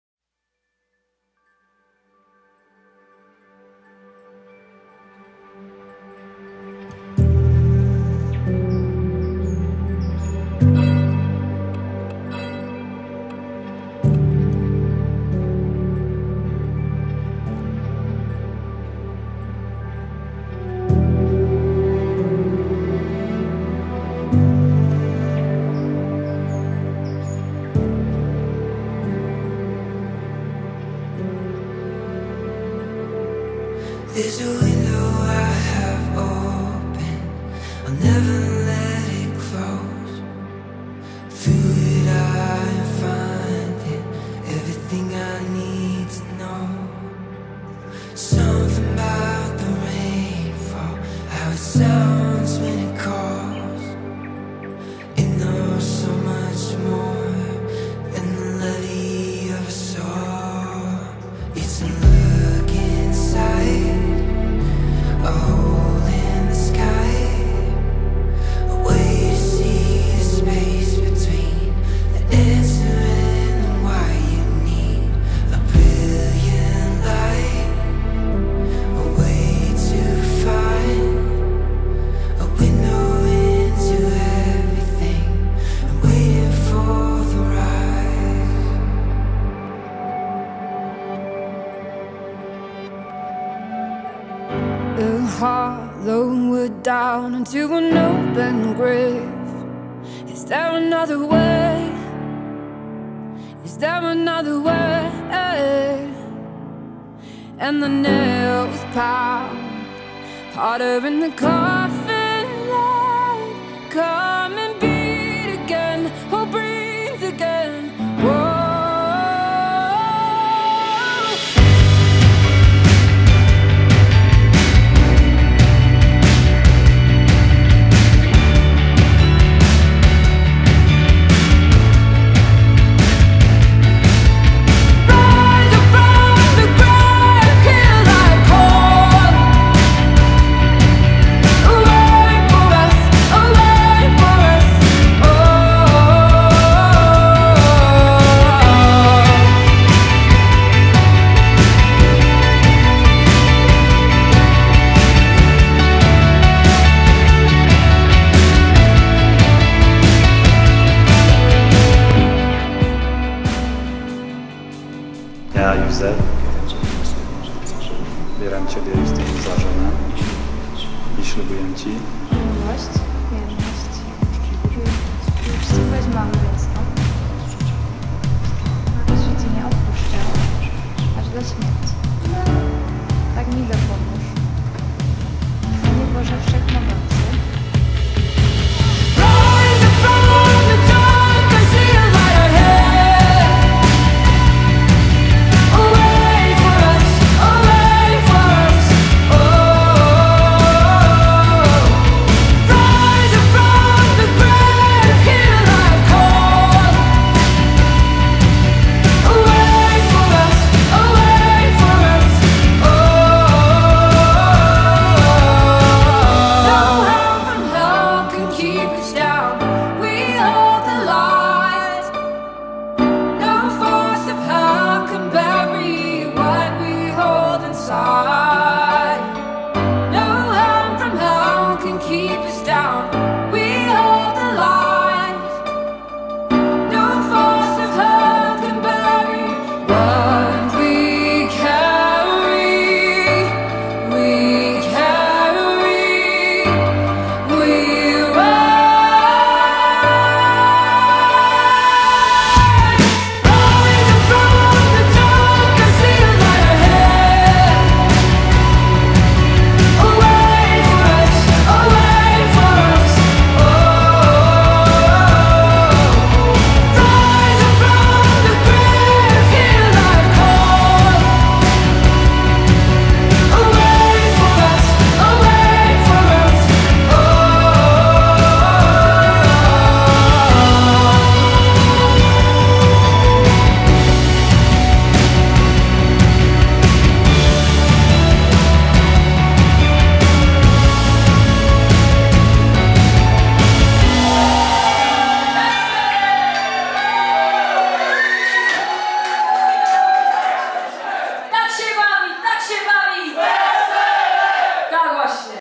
Teledysk ślubny | Justyna i Józef | Wesele w górach | 203 produkcje
Zapraszamy na ślub i wesele w górach.
Miejsce: Istebna
Zespół: Cantare Band

Zdjęcia i montaż:
Łukasz Polok
CHCESZ MIEĆ SUPER PAMIĄTKĘ ZE ŚLUBU I WESELA,